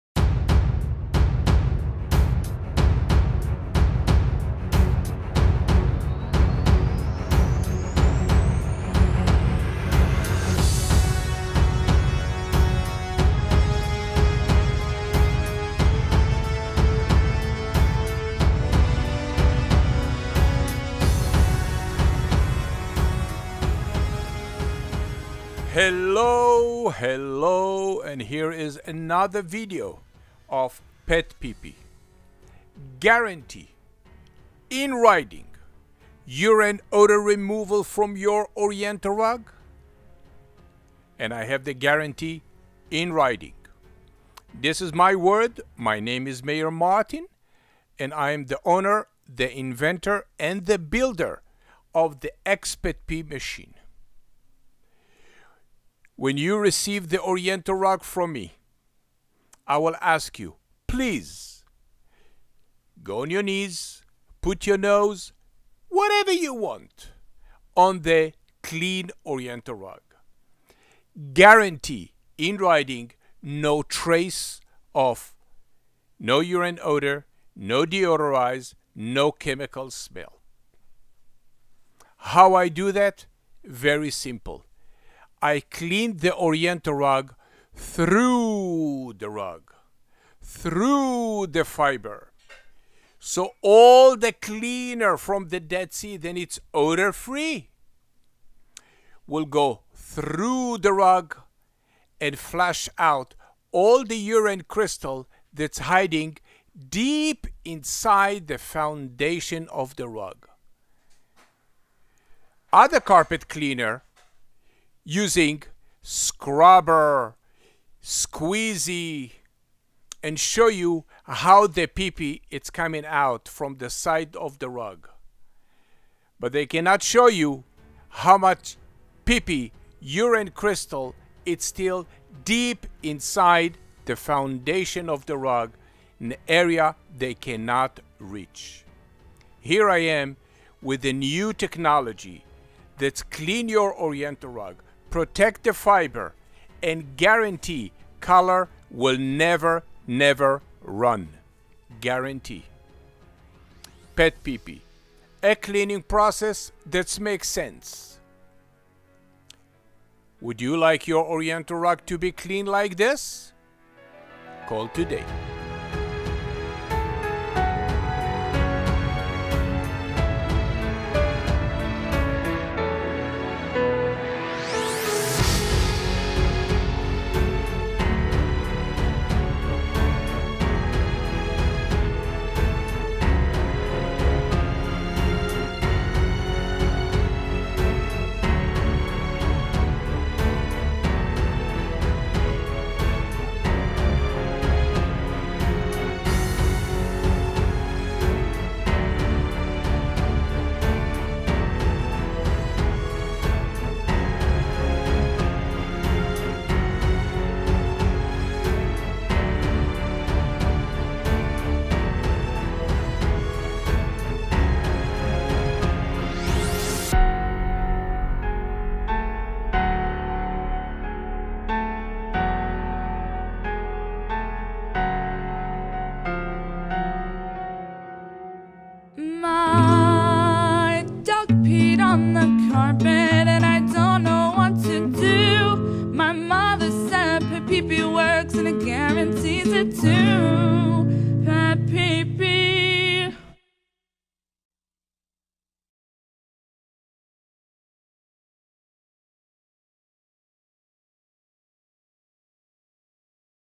A company that's guarantee urine odor removal from Oriental rug
PetPeePee company the name said it all.

Nationwide service with a guarantee in writing urine odor removal naturally, by using the odor-free cleaning product from the Dead Sea.

The new technology (the XpetPee) that's clean your Oriental rug through is the only method to eliminate the urine crystal odor from deep inside the foundation of the rug.

The movement of the cleaner on the top of the rug, the section through the rug, will ensure the Oriental carpet will be clean free from urine odor/crystal in the color will never run.

PetPeePee a cleaning process that's makes sense.

PetPeePee company specializes in Cleaning and Removing the dog cat urine odor from Oriental rug or Drapery. By using the Natural Dead Sea Cleaner And the XpetPee machine, we provide a 100% guarantee in writing urine odor removal completely permanently without masking or using the deodorizer or enzyme.